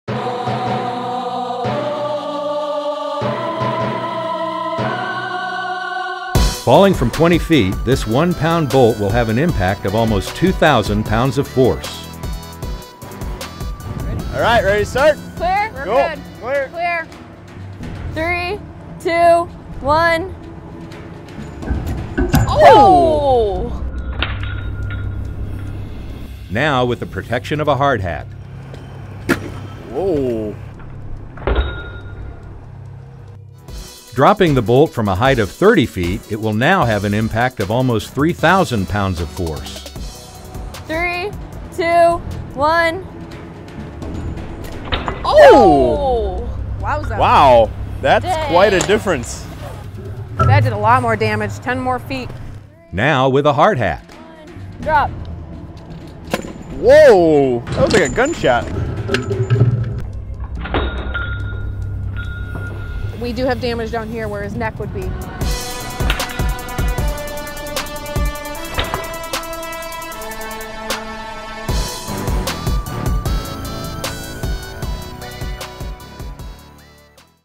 The Dropped Object Experiment | 1 LB Bolt, 20' vs. 30' | Safety Week
Falling objects can have huge consequences. In this video, we explore just how big a difference 10 feet can make when dropping a 1 pound object.

Transcript
FALLING FROM 20 FEET, A 1 POUND BOLT WILL HAVE AN IMPACT OF ALMOST 2,000 POUNDS OF FORCE.

"Alright, ready to start?"
"We're good to go. Clear."
"Clear. 3-2-1."

NOW WITH THE PROTECTION OF THE HARD HAT.

"Wow"

DROPPING FROM A HEIGHT OF 30 FEET, IT WILL NOW HAVE AN IMPACT OF ALMOST 3,000 POUNDS OF FORCE.

"3-2-1 dropping"
"Wow. That's quite a difference."
"That did a lot more damage. Ten more feet."

NOW WITH A HARD HAD.

"Dropping."
"Whoa. That sounded like a gunshot."
"We do have damage down here where his neck would be."

END